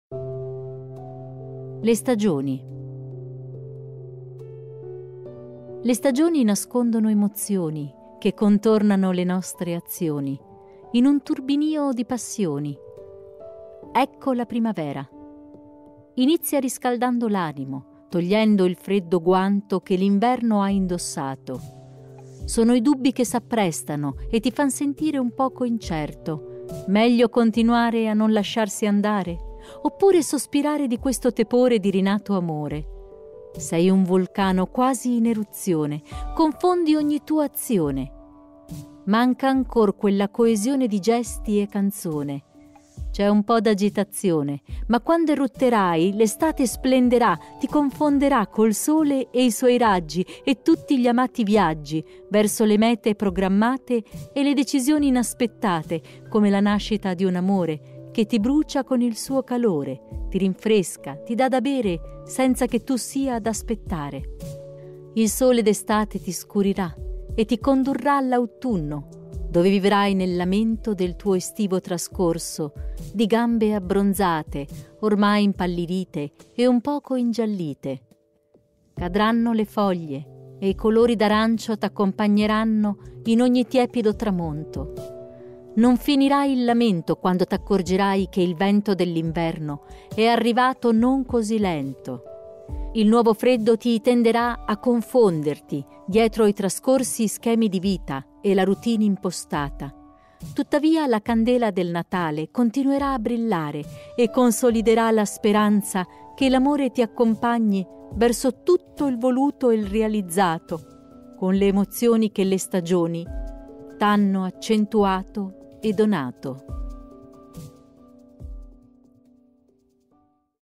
Giulia Bedini-05 Le stagioni
(Massa, 19/09/1985) Ha conseguito la laurea magistrale presso la Luiss “Guido Carli” di Roma. Per lei la poesia è raccontare una storia facendo rivivere al lettore l’emozione scaturita dal suo dolore. Ha partecipato solo ad alcuni concorsi territoriali rientrando sempre tra le prime posizioni.